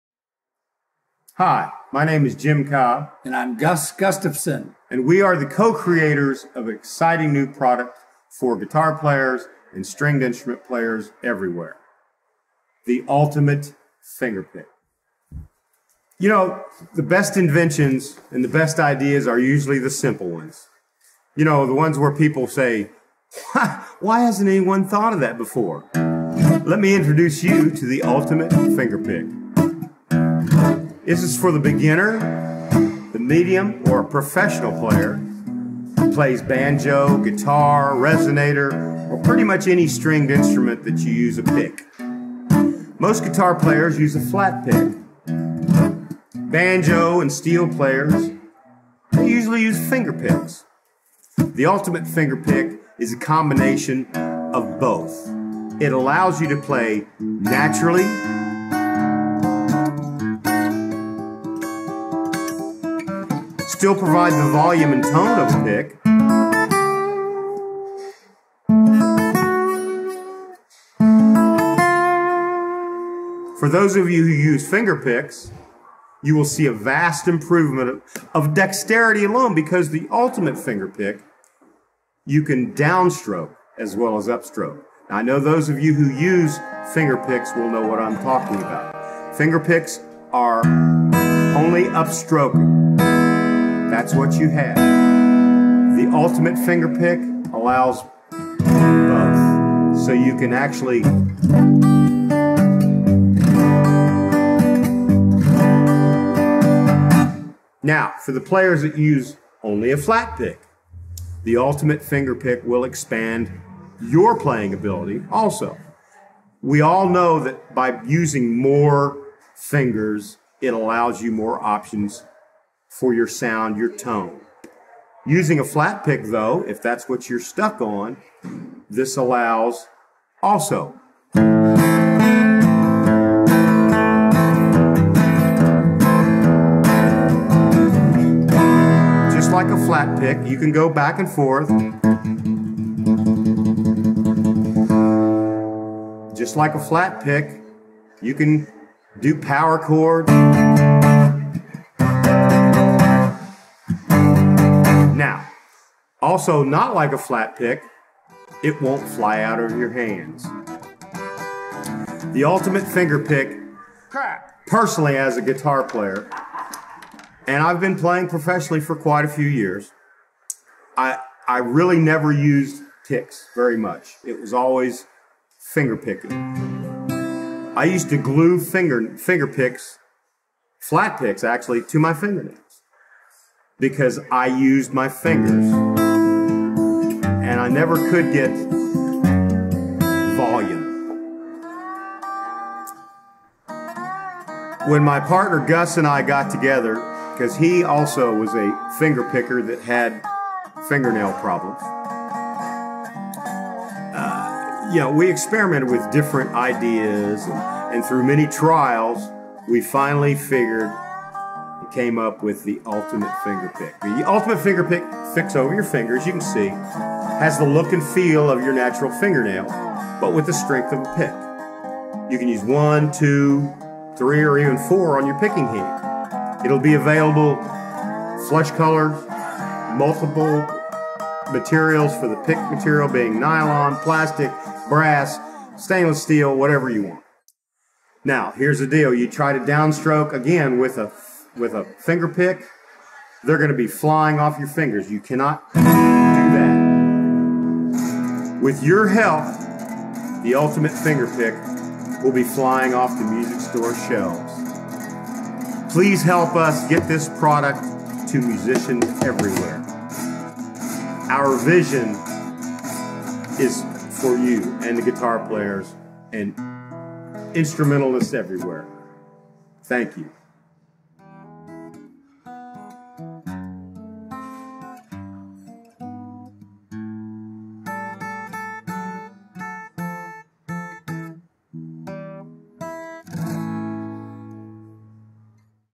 The Ultimate Fingerpick; Original Infomercial prior to the 1st production run...
This was the initial "infomercial" for the Ultimate FingerPick, with only the orange prototypes existing at the time...we hadn't even gone into production yet. [the orange ones were never mass produced]
This video remains to honor my good friend and co-inventor, Jim Cobb.